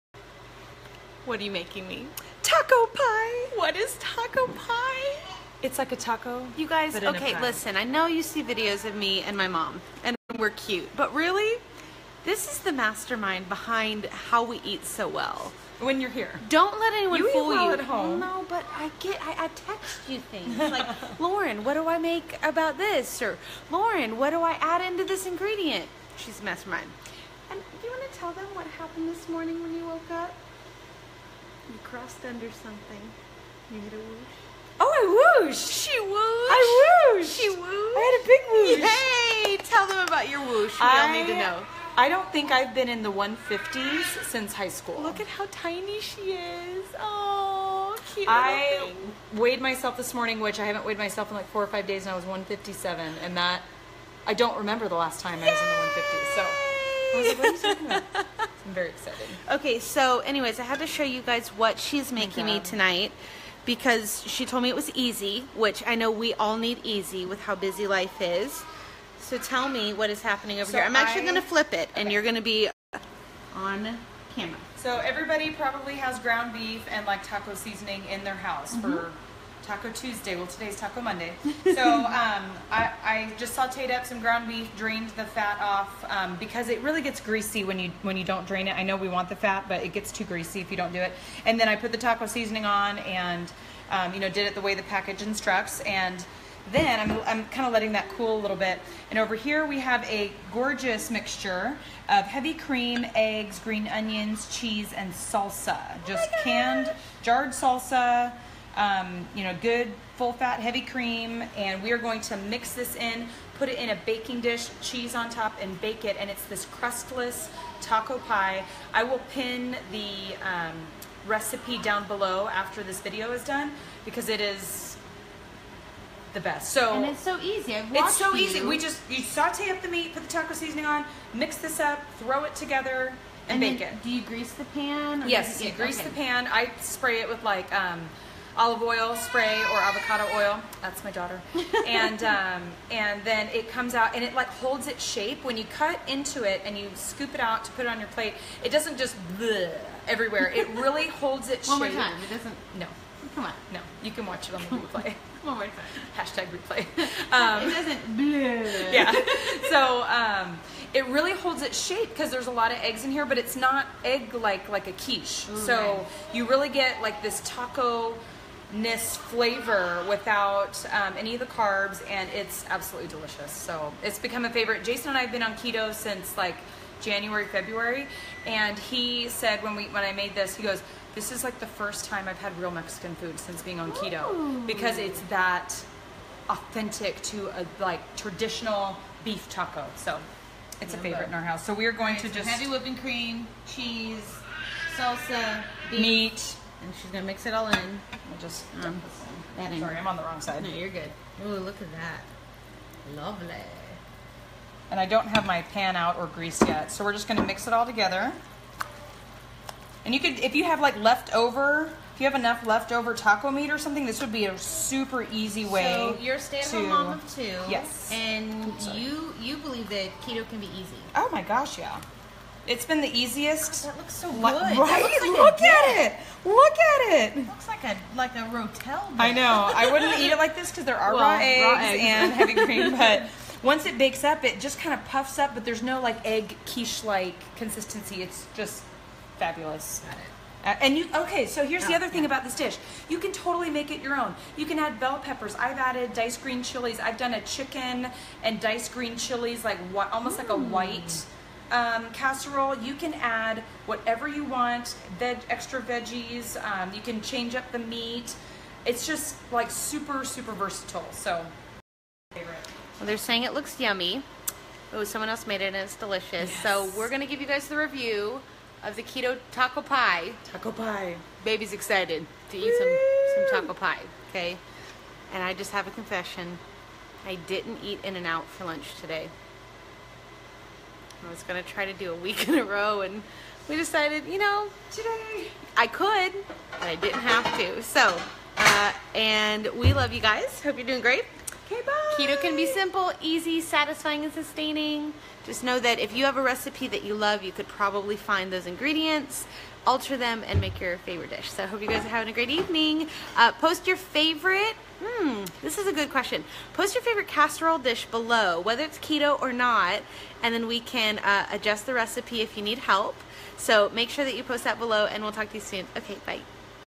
Super Easy Keto Taco Pie
Super easy Keto Taco PIE!  One of the most beloved keto recipes that we adapted So versatile and can be made into muffins for easy lunches